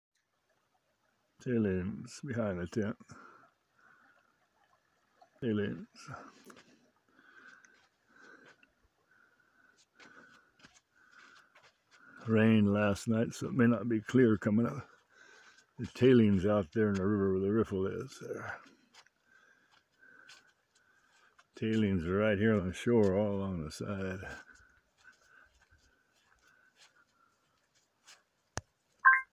Dredging for gold on the Mosquito Fork of the Fortymile river.
Gold dredging on the rivet.